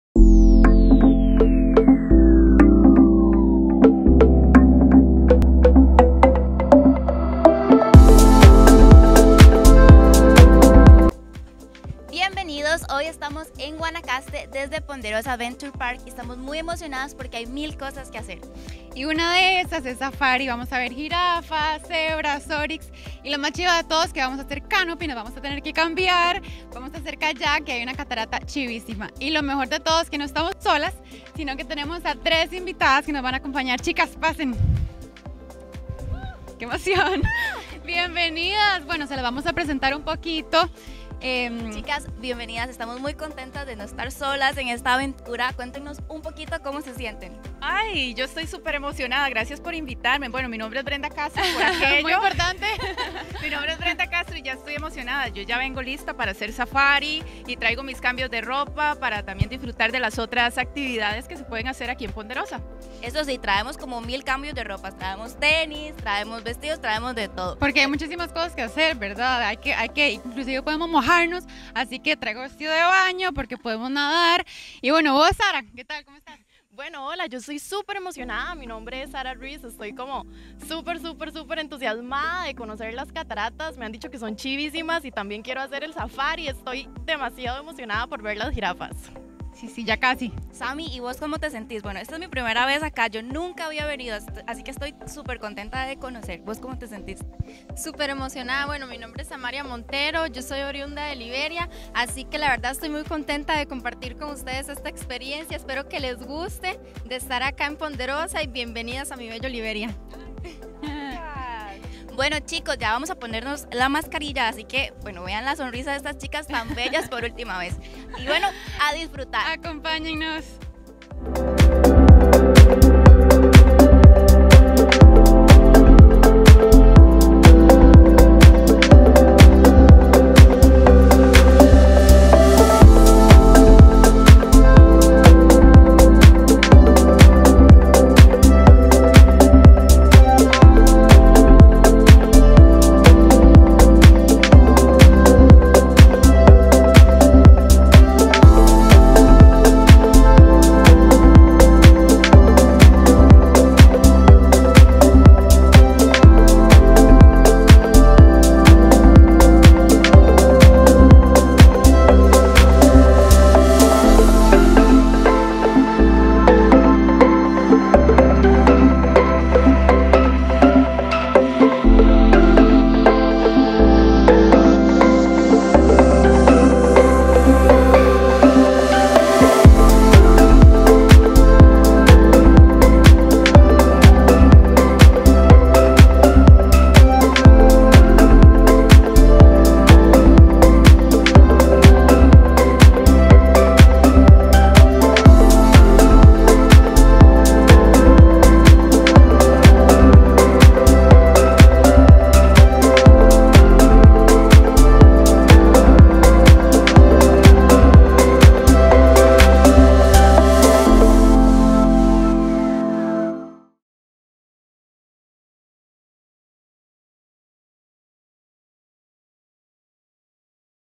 Experiencia del viaje donde fuimos invitados. Ponderosa Adventure Park
Ponderosa Adventure Park